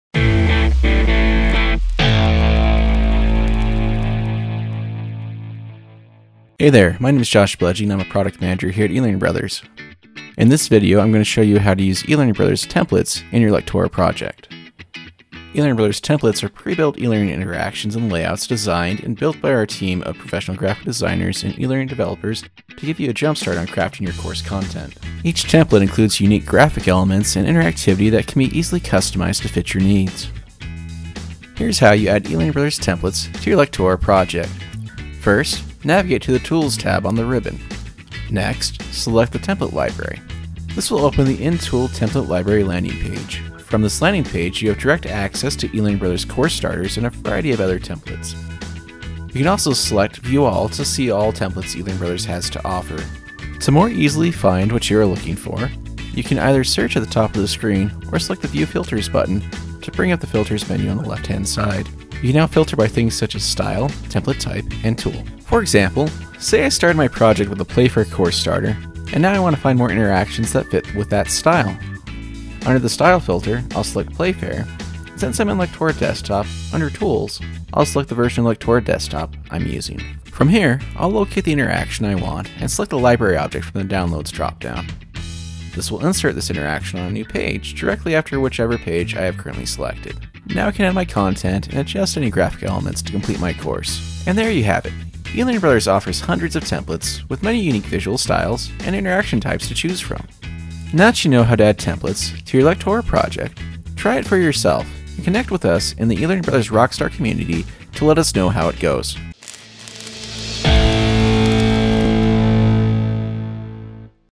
Using Templates in Lectora - Lectora Online & Lectora Desktop
In this video you will learn how to add eLearning Brothers templates to your Lectora project, without ever leaving Lectora.